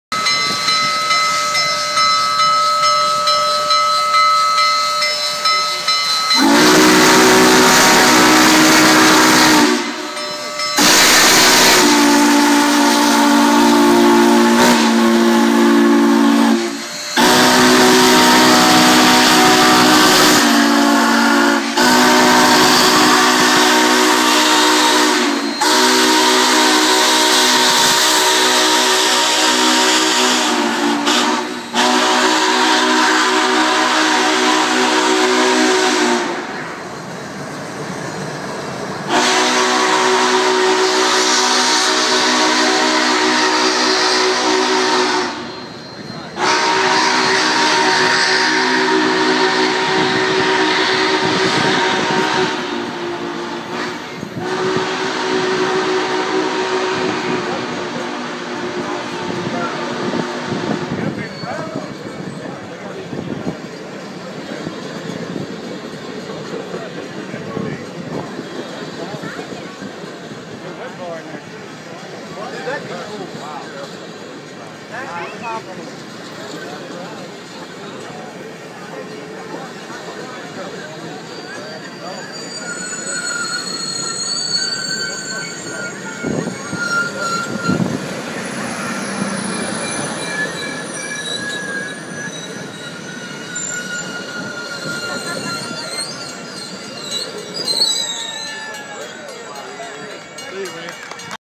150 years of up
Celebrating 150 years the union Pacific steam engine driven train.